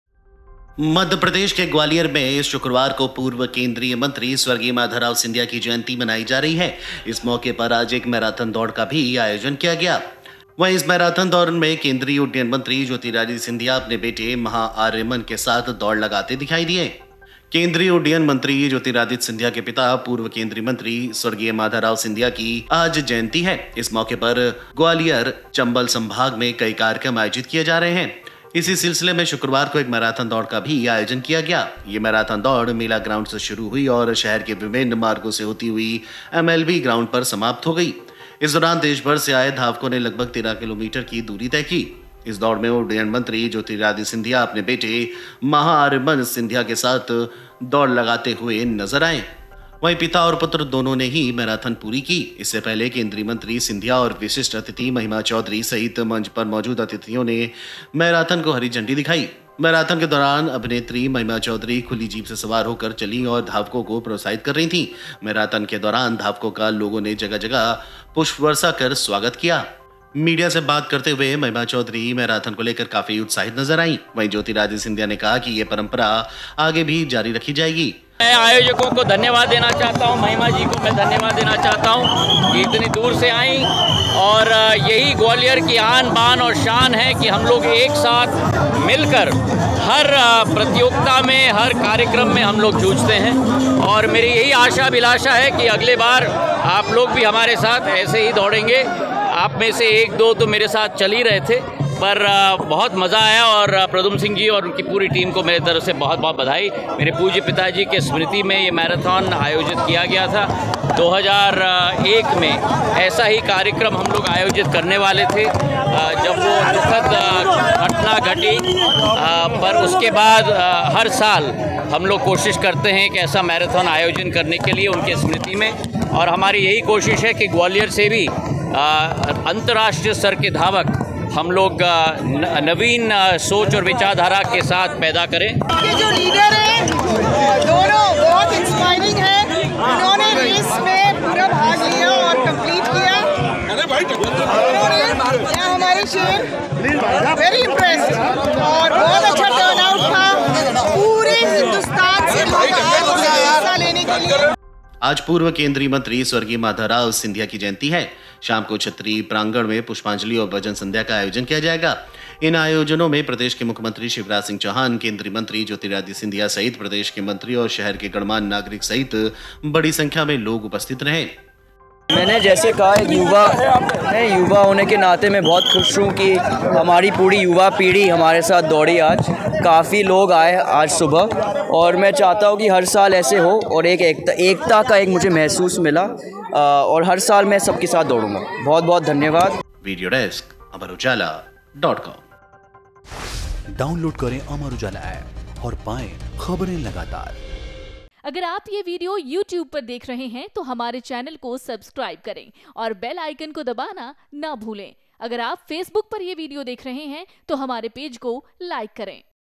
Gwalior News: माधवराव की स्मृति में राज्य मैराथन में बेटे संग दौड़े ज्योतिरादित्य सिंधिया | MP News
मध्यप्रदेश के ग्वालियर में आज शुक्रवार को पूर्व केंद्रीय मंत्री स्वर्गीय माधवराव सिंधिया की जयंती मनाई जा रही है। इस मौके पर आज एक मैराथन दौड़ का भी आयोजन किया गया। इस दौरान केंद्रीय उड्डयन मंत्री ज्योतिरादित्य सिंधिया अपने बेटे महा आर्यमन के साथ दौड़ लगाते दिखे।